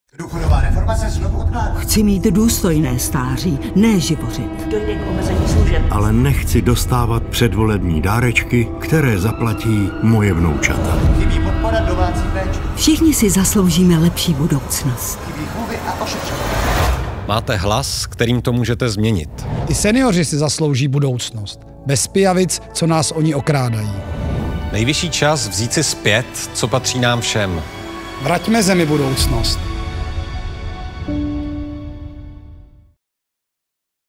I senioři si zaslouží lepší budoucnost
I senioři si zaslouží lepší budoucnost. Bez pijavic, co nás o ní okrádají. Nejvyšší čas vzít si zpět, co patří nám všem. Už 8. a 9. října.